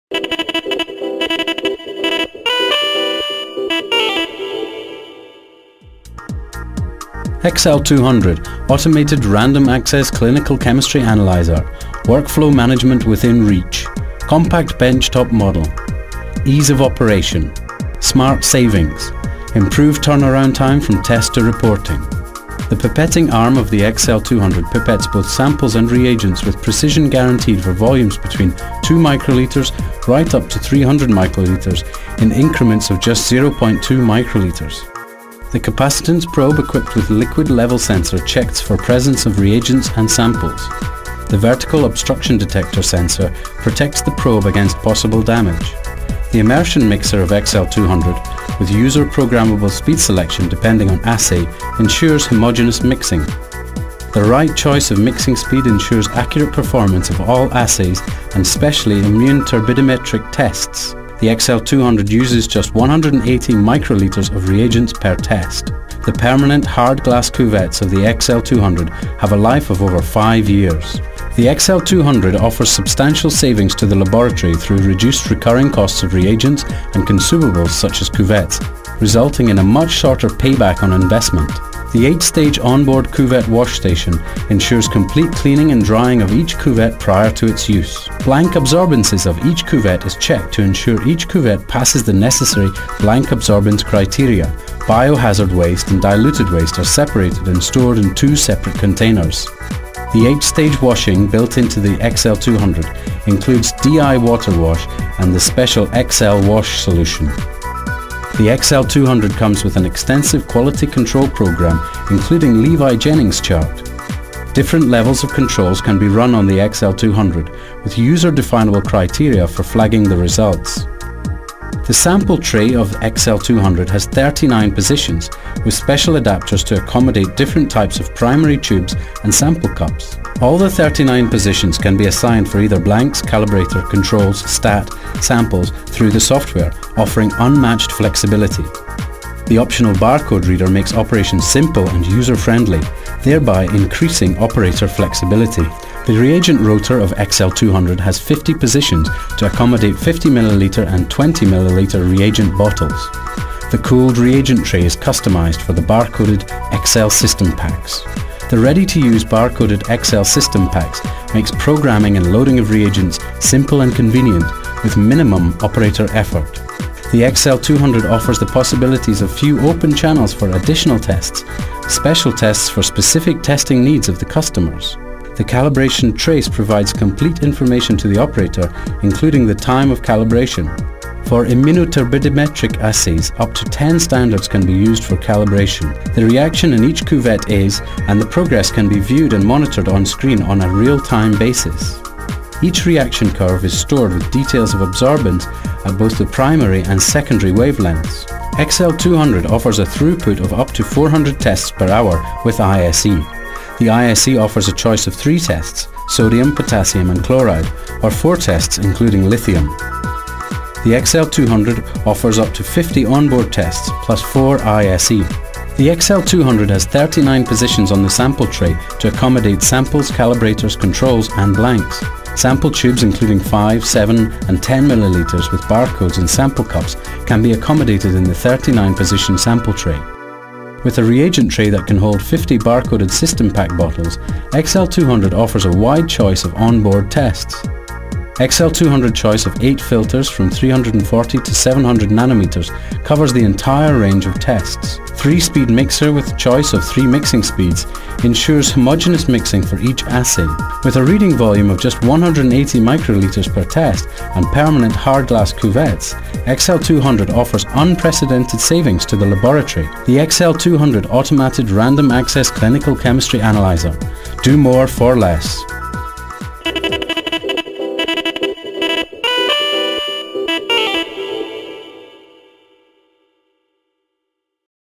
BORPOL oferta: XL200 automatyczny analizator biochemiczny firmy Erba Mannheim biochemia
Automatyczny analizator o swobodnym dostępie, przeznaczony do średnich laboratoriów. Zoptymalizowany do pracy na odczynnikach firmy BioSystems, z dostępną linią barkodowanych odczynników dedykowanych.
Analizator o wydajności 200 badań/godzinę (bez ISE), 400 badań/godz. (z ISE).
Polskie, przyjazne i wielofunkcyjne oprogramowanie użytkownika.

Zakres testów
• enzymy, substraty, pierwiastki, substancje nieorganiczne, inne
• testy turbidymetryczne
• testy wyliczeniowe, na wynik których może składać się aż 5 wyników testów  składowych -- możliwość zaprogramowania przez użytkownika własnych  wzorów dla testów wyliczeniowych
• bezpośredni pomiar potencjometryczny elektrolitów Na, K, Cl, Li (opcjonalnie, z możliwością wyłączenia litu)